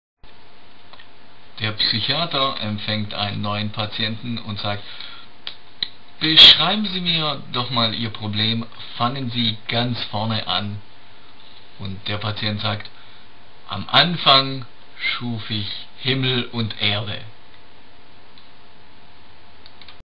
Witze- Psychiater
Toller Witz von unserem Spitzenwitzeerzähler Soko
Witz "Psychiater"